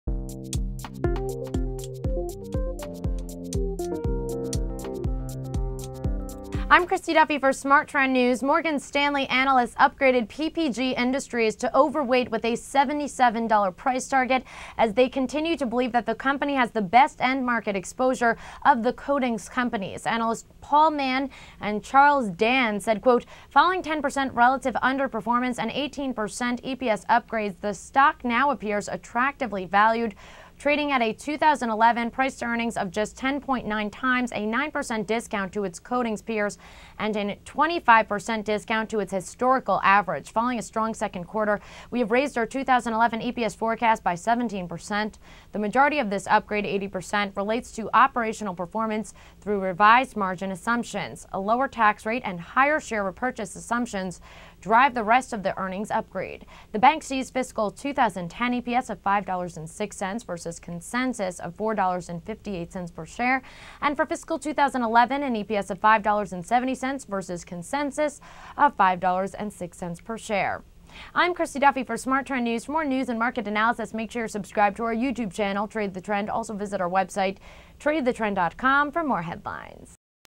Analyst Insight: Morgan Stanley Ups PPG Industries To OW
Morgan Stanley analysts upgraded PPG Industries (NYSE:PPG) to overweight, with a $77 price target, as they continue to believe that the company has the best end-market exposure of the coatings companies.

Analysts Paul Mann and Charles Dan said, "Following c. 10% relative underperformance and 18% EPS upgrades, the stock now appears attractively valued, trading at a 2011 P/E of just 10.9x, a 9% discount to its coatings peers and an 25% discount to its historical average. Following a strong Q2, we have raised our 2011 EPS forecasts by 17%. The majority of this upgrade (80%) relates to operational performance through revised margin assumptions. A lower tax rate and higher share repurchase assumptions drive the rest of the earnings upgrade."

The bank sees fiscal 2010 EPS of $5.06, vs. consensus estimates of $4.58 per share, and fiscal 2011 EPS of $5.70, vs. consensus estimates of $5.06 per share.